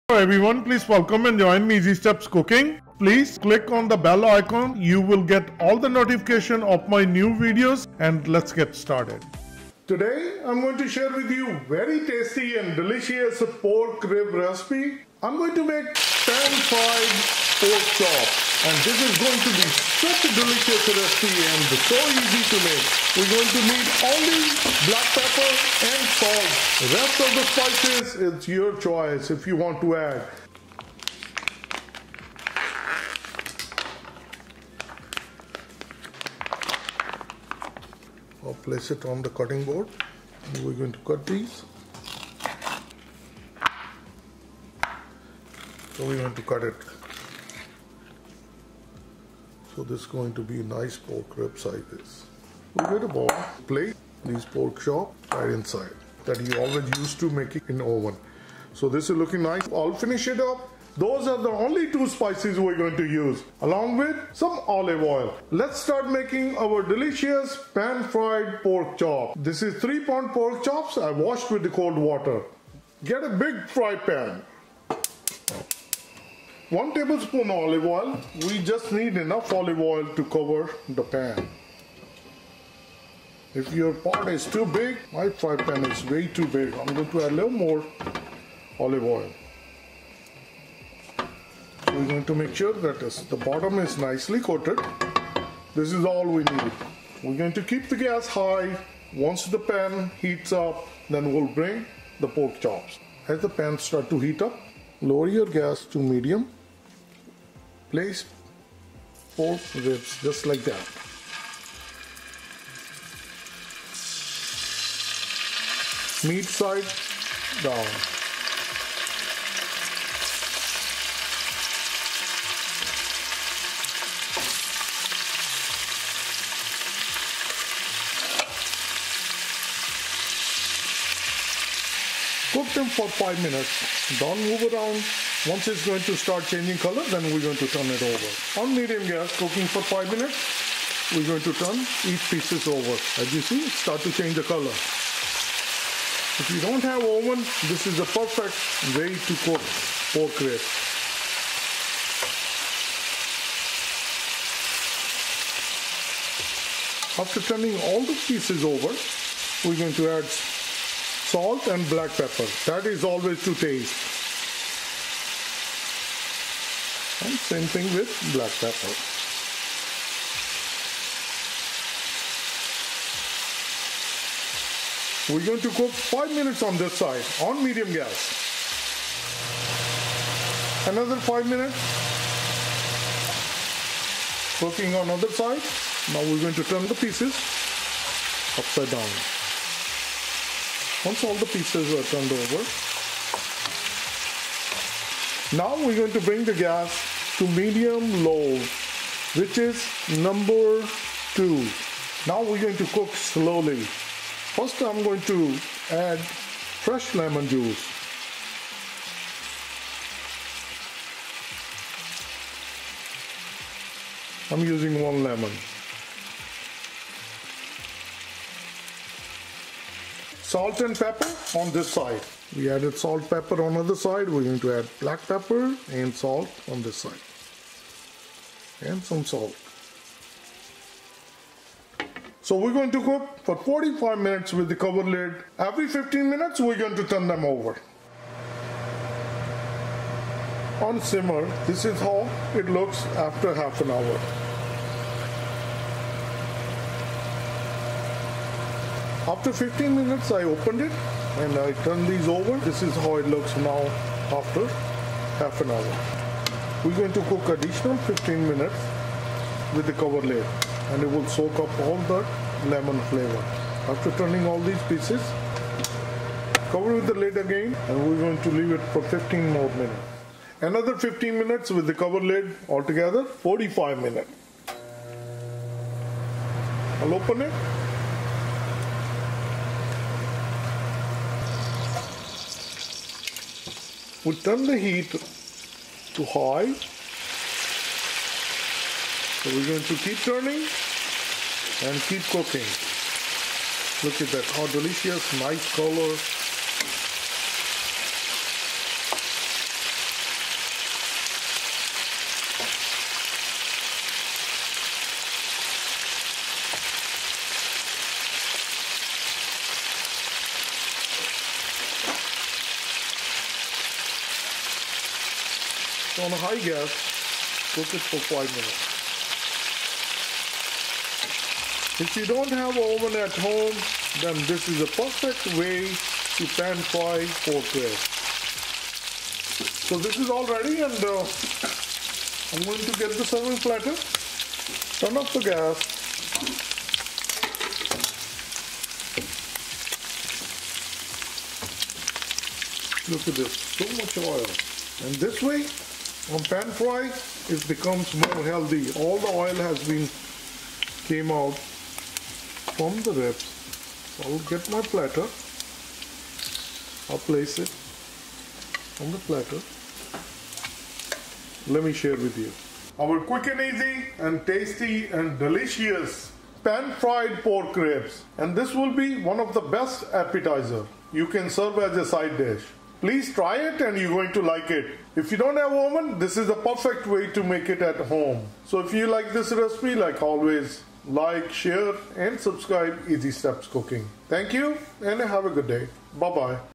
How To Cook Pan Fried Pork Loin Back Ribs Juicy, Tender, and Moist without Oven
Ingredients List:
3 lbs. Pork Loin Back Ribs
Pink Salt
Black Pepper
Lemon Juice